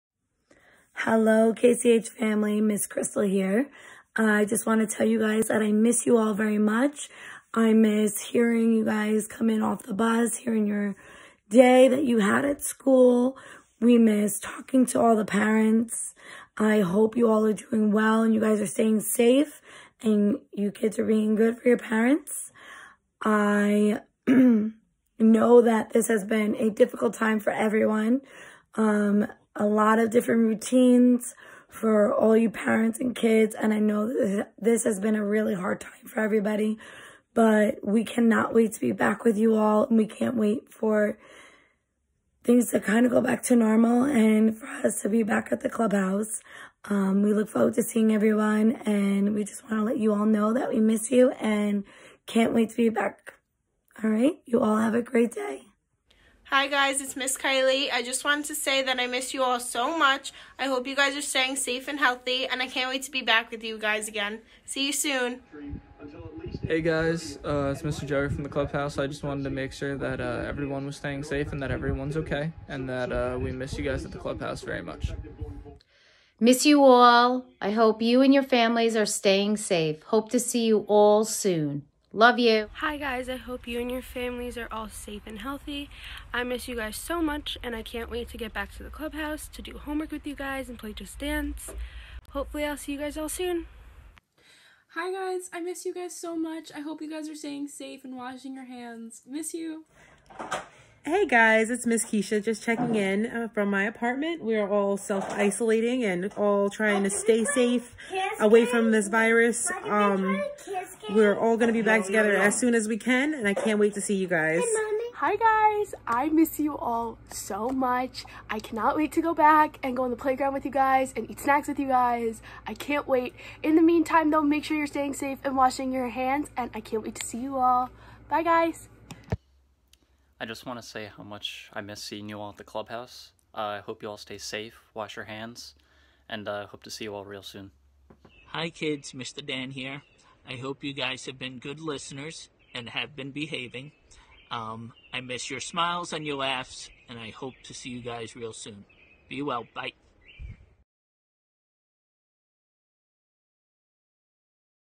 We Miss You (part two)
WE MISS YOU! We all hope that you are staying healthy and safe during this time! Here are some of the Crew from the Clubhouse coming to check in on our kids and their families! We hope to see you soon!